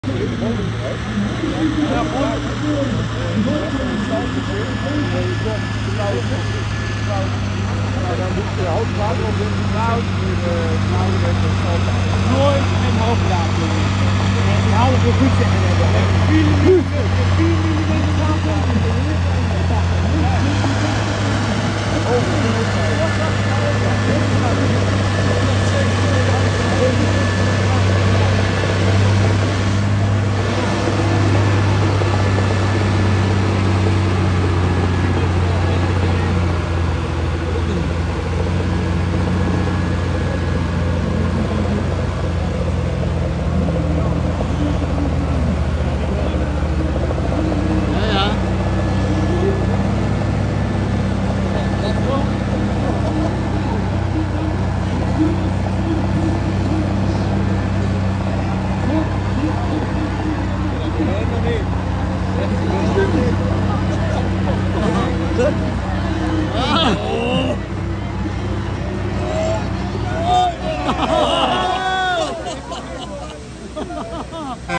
Valtra 8950 trekkertrek terwolde 9 ton
2e trek voor de valtra man goed voor een dikke 1e plek 9000 kg klasse